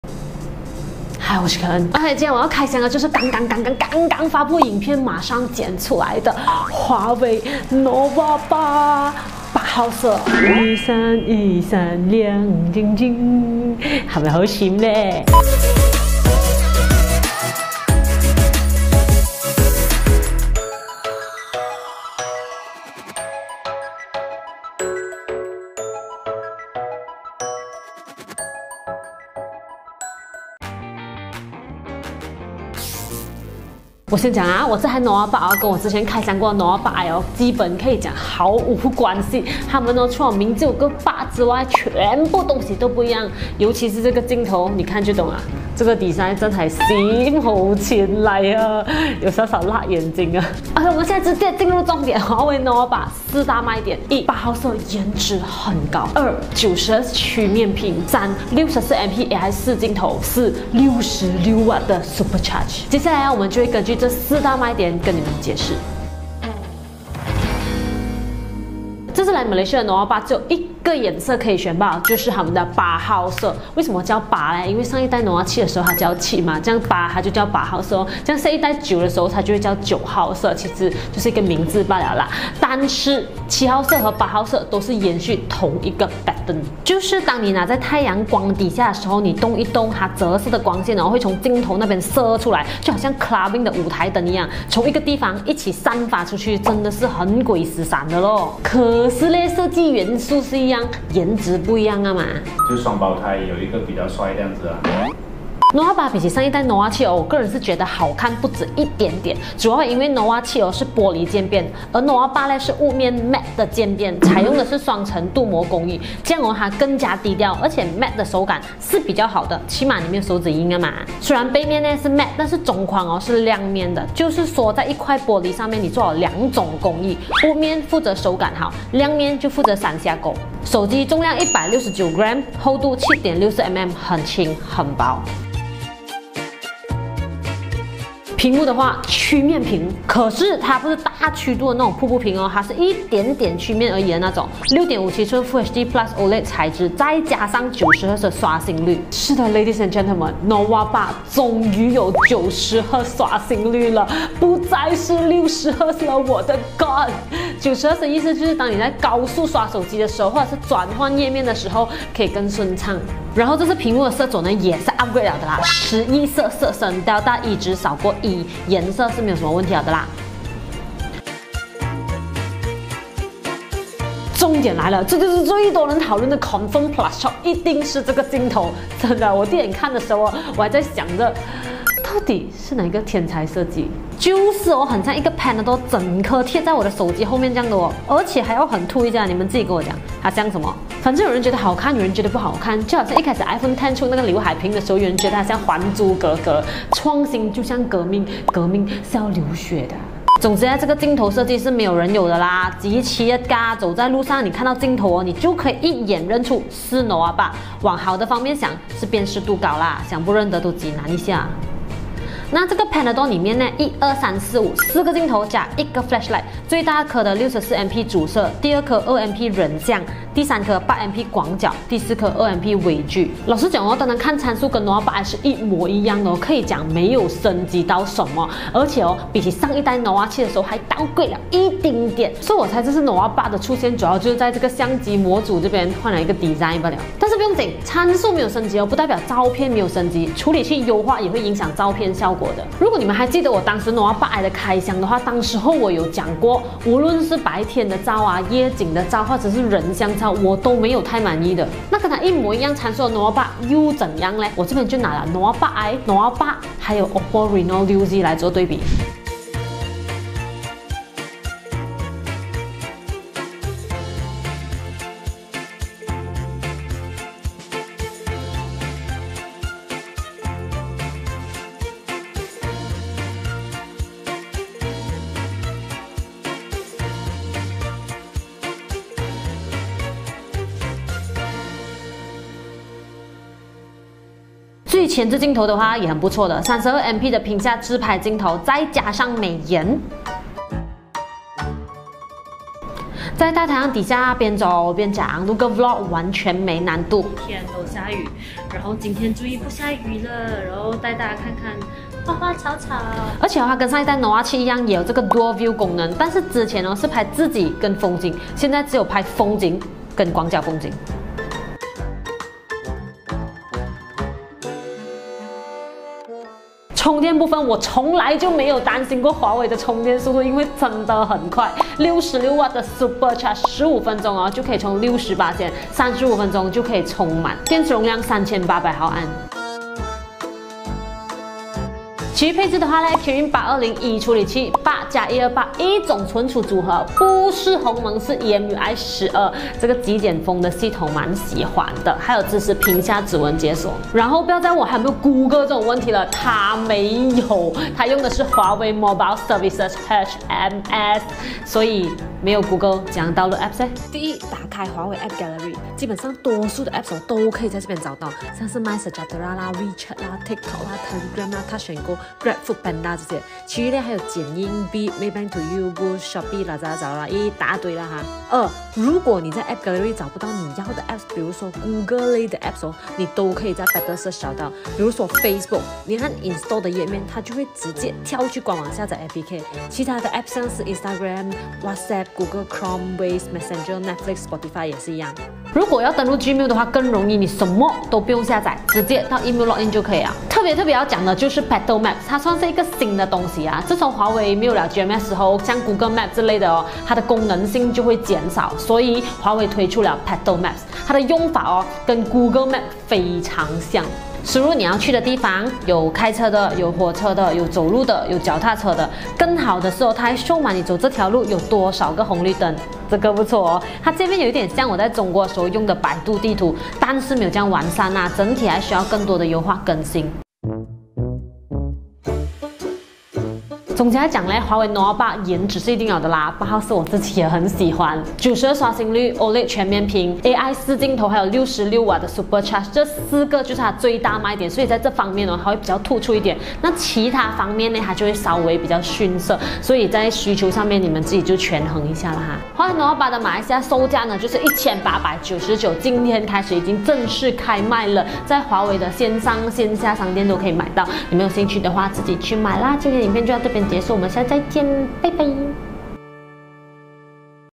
我拿到了华为Huawei nova 8！8号色太闪了，RM1899值不值得买？
今天nova 8正式在马来西亚开卖啦!!
马上来看开箱测评！
里面还有nova 8 vs 8i vs OPPO Reno 6Z拍摄样张对比哦~